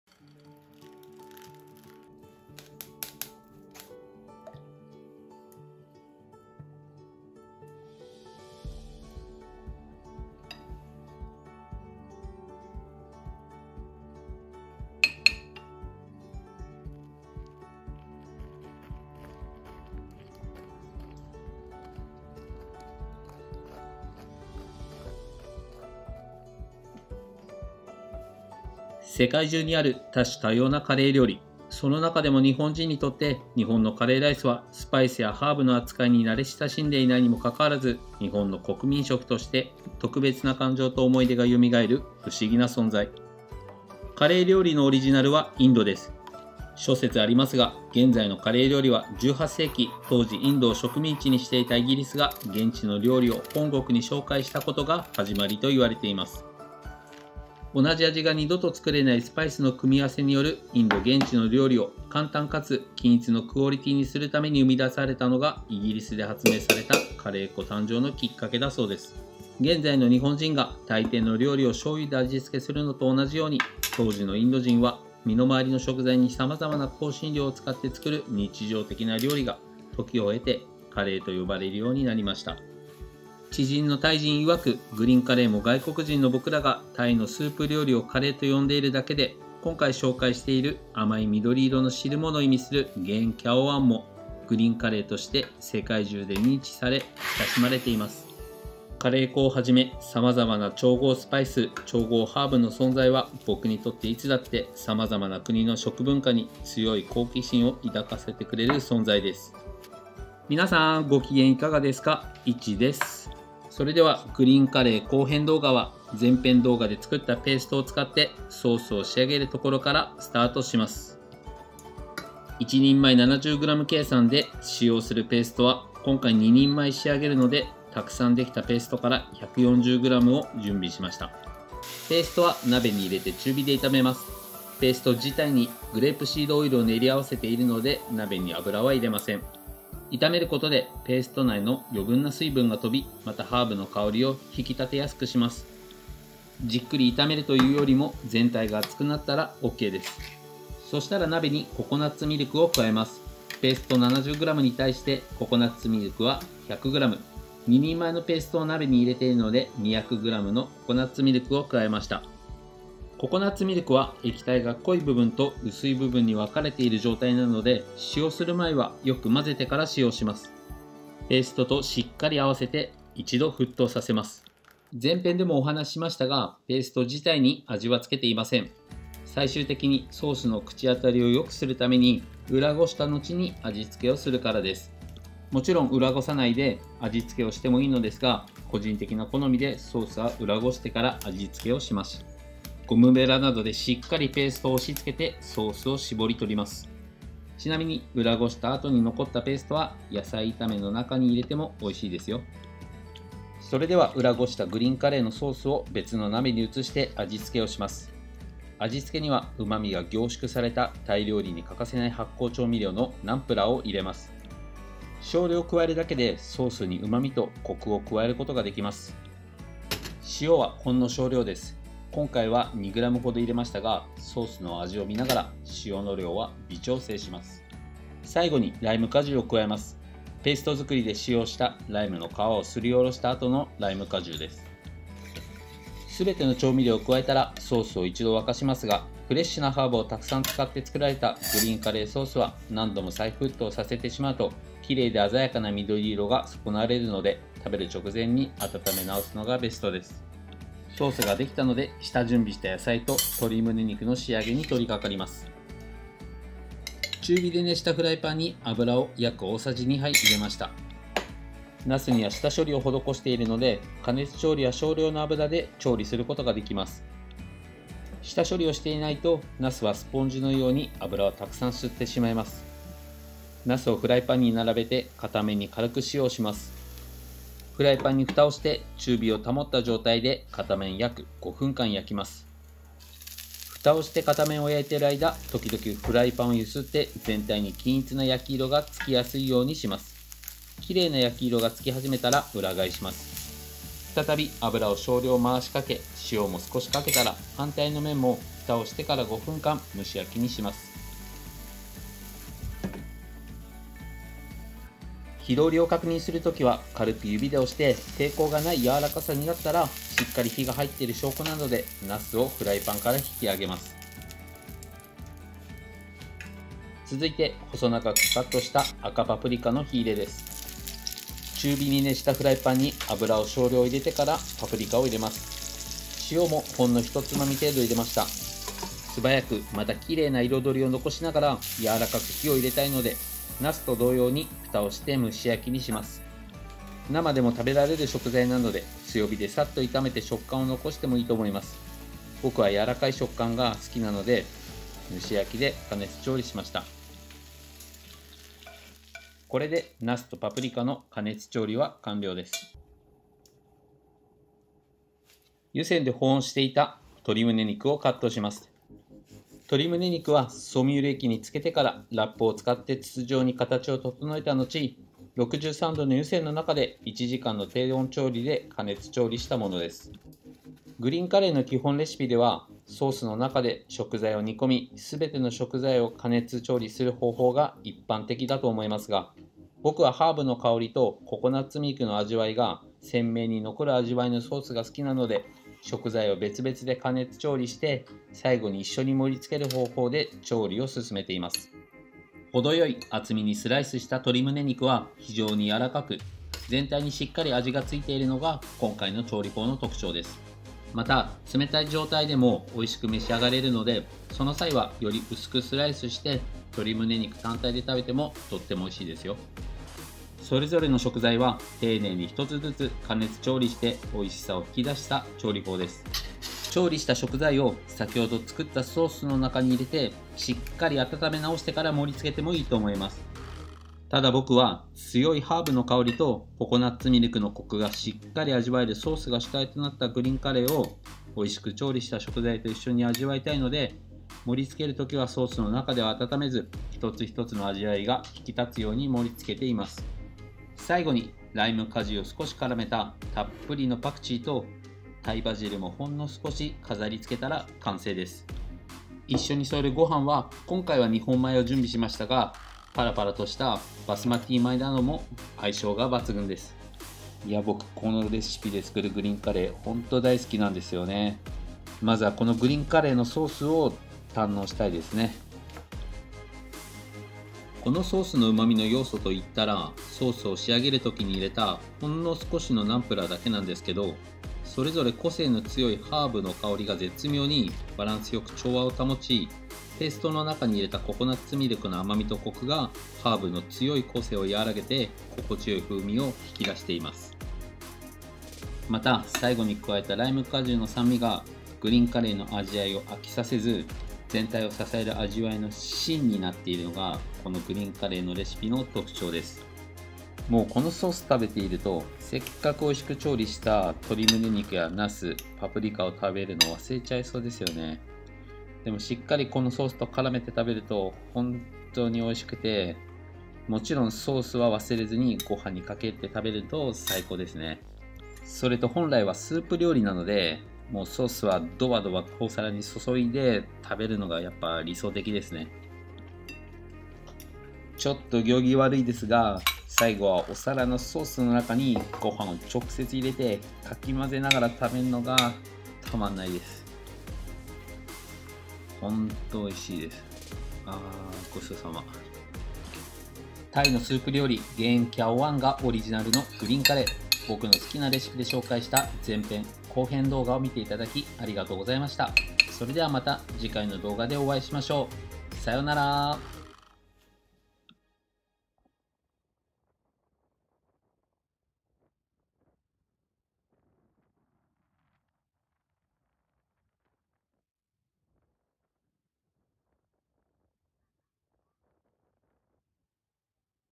【グリーンカレー②】￼11種類のハーブとスパイスで作るカレーペーストがたまらなく旨味・後編 / Poulet Thaï Curry Vert
タイのスープ料理、ゲーン・キャオ・ワーンがオリジナルのグリーンカレー後編動画。

前回の前編動画で紹介したグリーンカレーペーストを使ってソースを仕上げます。

ソースと一緒に食べる鶏胸肉の下処理野菜の下準備は、前編動画で紹介しています。

✅グリーンカレーソースの材料と分量（２人前）

グリーンカレーペースト　１４０ｇ
ココナッツミルク　２００ｇ
ナンプラー　１０ｇ
塩　２ｇ
ライム果汁　５ｇ

✅前回動画・グリーンカレーペーストの材料
【材料と分量】

グリーンカレーペーストの分量￼(20人前) ￼

ワケギ　　 ５００ｇ
青唐辛子　１００ｇ
ホーラパー（タイバジル）１束
ガランガル(タイ生姜)　  ４５ｇ
パクチー　２束（１００〜１３０ｇ）
レモングラス　５０ｇ
にんにく　３０ｇ
コブミカンの皮　２個分
ライムの皮　　３個分
クミンパウダー　１５ｇ
コリアンダーシード　１５ｇ
グレープシードオイル　５００ｇ